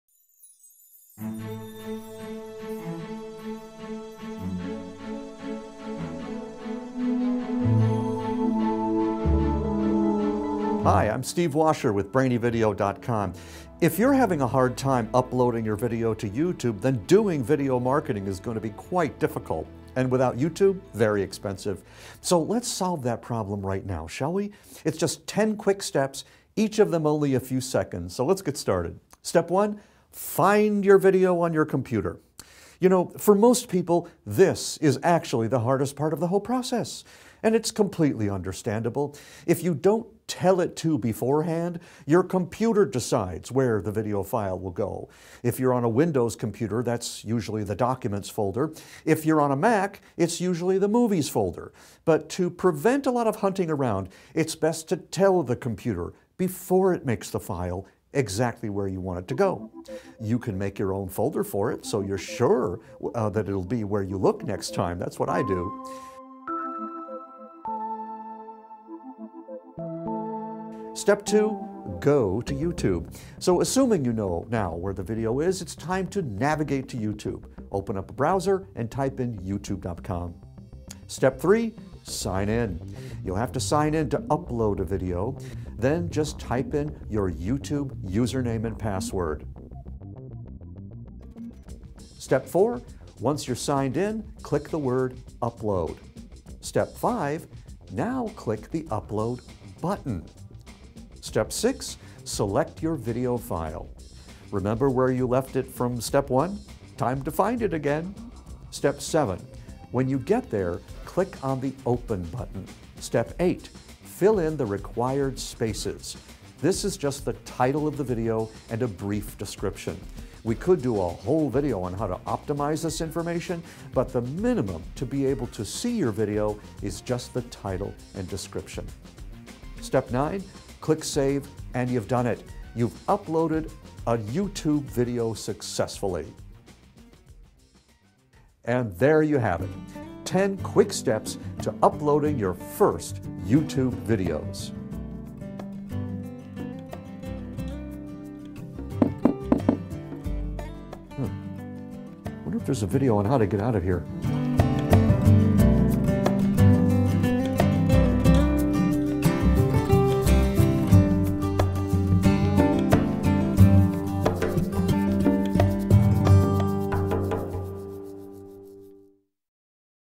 How To Upload a Video to YouTube
Knowing how to upload a video to YouTube might be one of the givens of video marketing, but for many it's a stumbling block. If you want to market your business using video online and can't use YouTube, you're truly one of the "have-nots."

So let's transform you into a video mogul right now.

0:29 Step 1: Find your video
1:11 Step 2: Go to YouTube
1:21 Step 3: sign in
1:32 Step 4: Click on "Upload"
1:37 Step 5: Click the Upload button
1:41 Step 6: Navigate to your file
1:48 Step 7: Click "Open"
1:53 Step 8: Fill in 2 blanks
2:10 Step 9: Save it!

That's it. But check out the very ending to make sure you don't get trapped in the YouTube twilight zone!